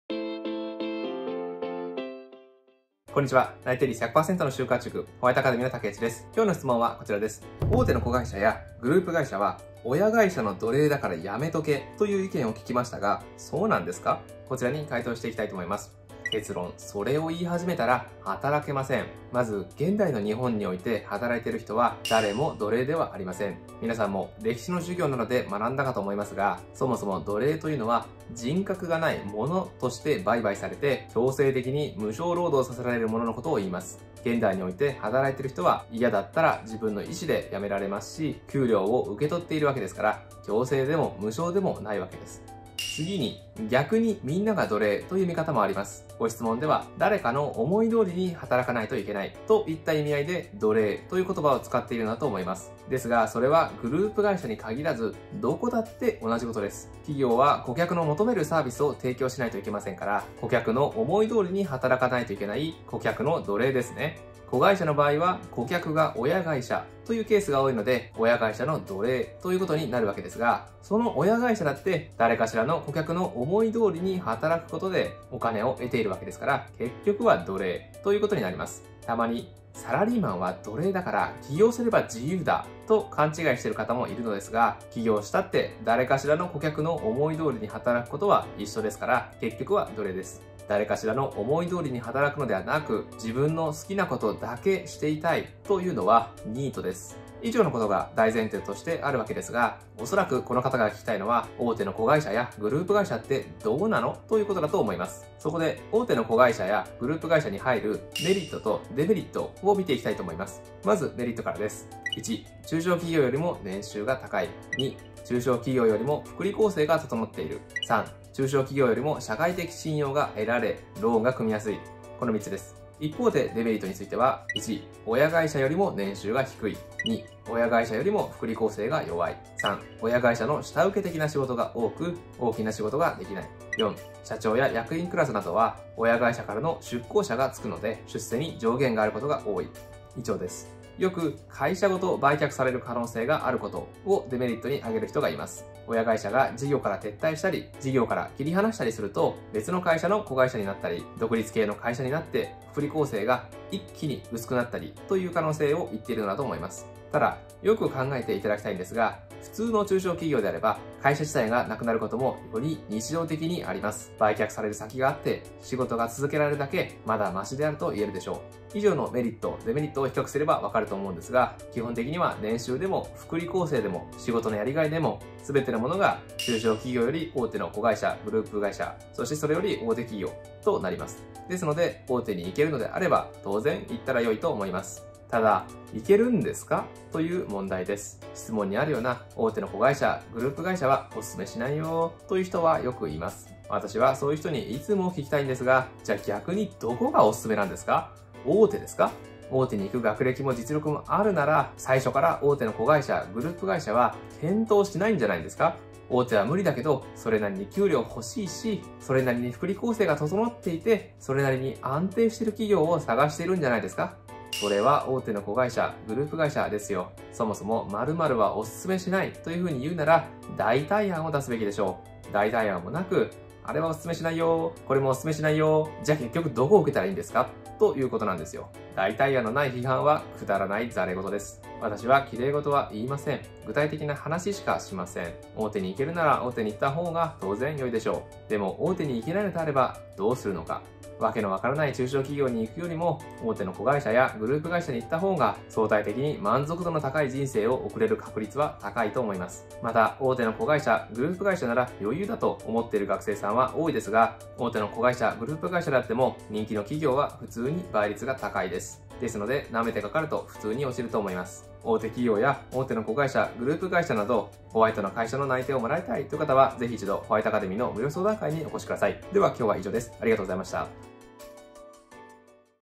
【就活】大手のグループ会社のメリットとデメリットを解説
●自分の就活力と想定内定先がわかる書籍『内定力診断』をプレゼント中！↓

●関連動画
・大手のグループ会社の見つけ方

【目次】
0:00 導入
0:19 誰も奴隷ではない
0:42 逆に誰もが奴隷である
1:37 メリットとデメリットについて
2:48 大手の子会社やグループ会社を目指すべき理由
4:21 最後に